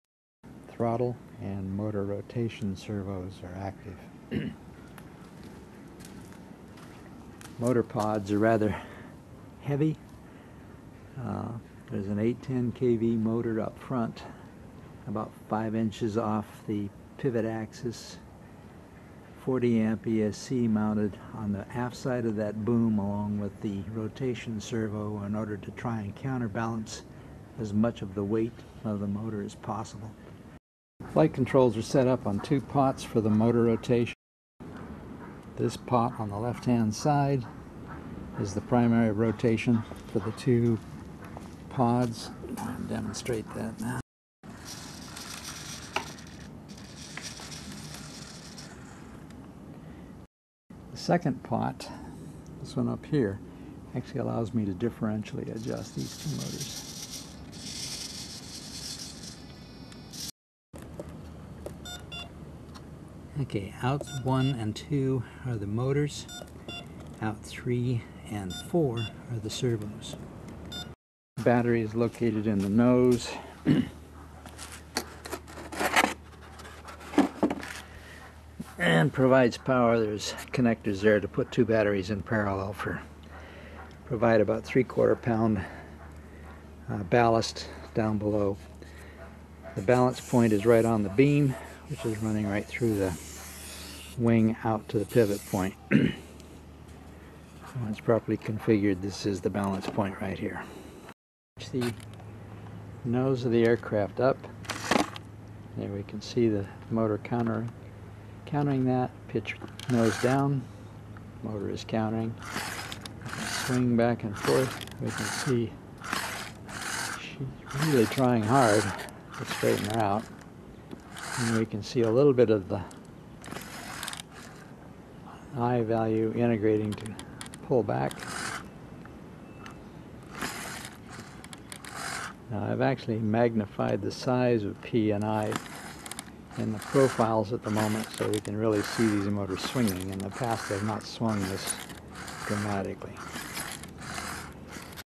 RC V22 Osprey motor pod control setup
two pots on transmitter are used to move the motor pods together for VTOL to FFF and differential for yaw control.  AutoLevel is active for dynamic stability on yaw and pitch.